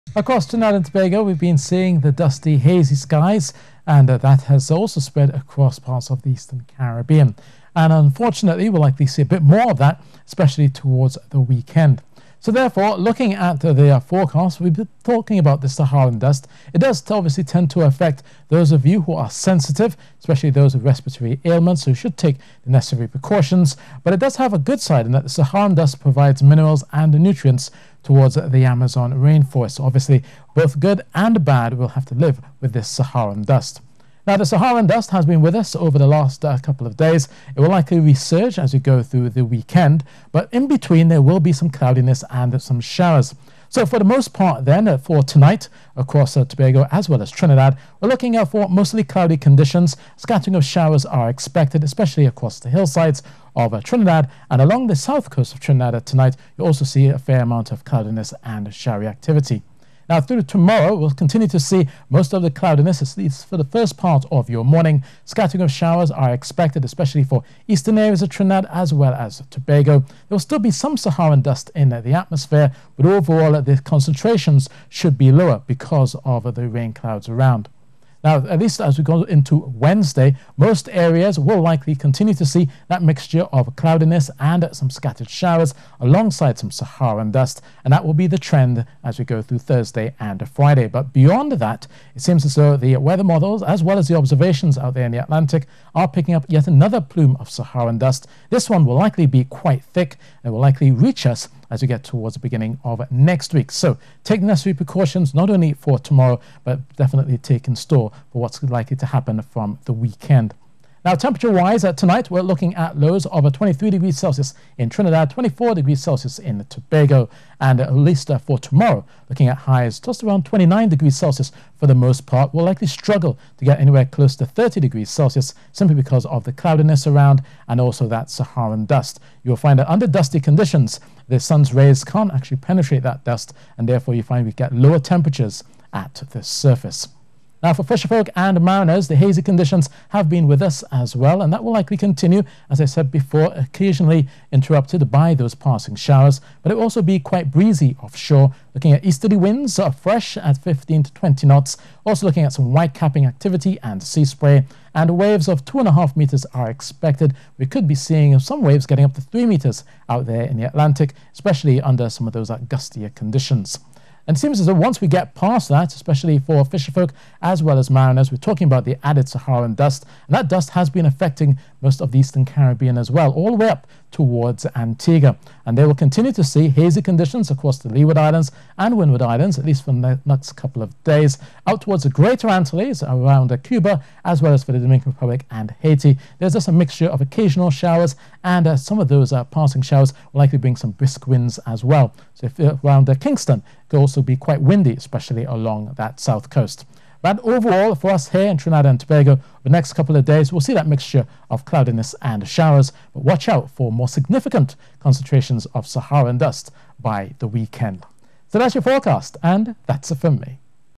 Weather Outlook - Monday February 22nd 2021
TTT Meteorologist Ean Wallace talks about more Saharan dust on the horizon.